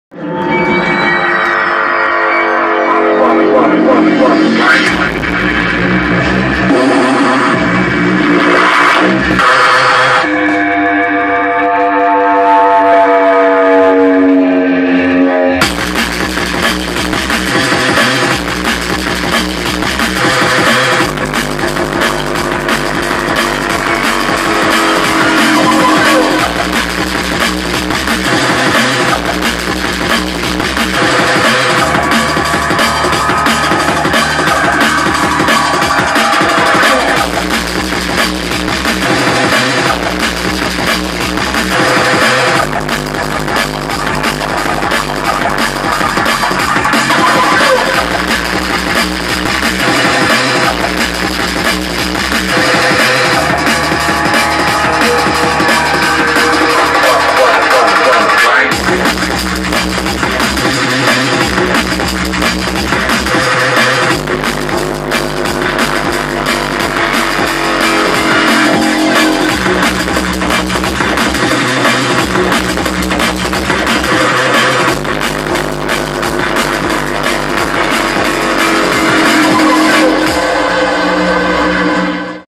KROT live visuals Ultravisual Gang VJ’s
Kirill Orlov aka KROT live visuals Ultravisual Gang VJ's
Kosmos Nash Festival 2016